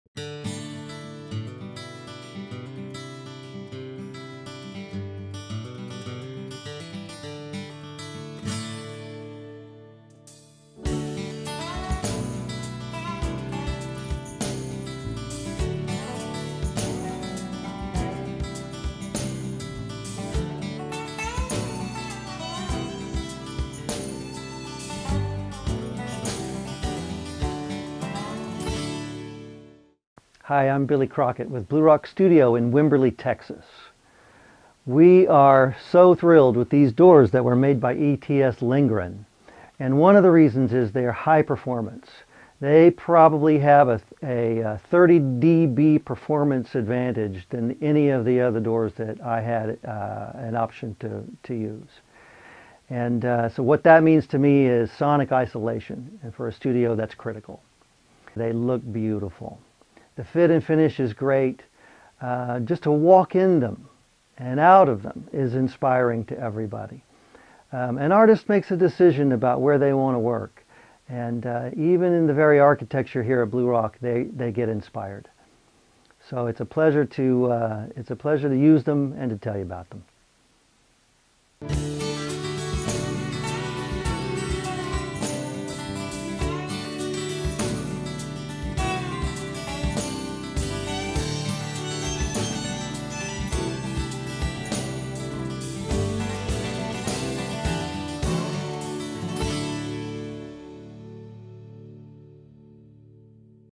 ETS-Lindgren SoundSecure™ Plug-in Acoustic Door
Billy Crockett introduces an ETS-Lindgren SoundSecure™ Plug-in Acoustic Door installed at Blue Rock Studio​ in Wimberly, Texas.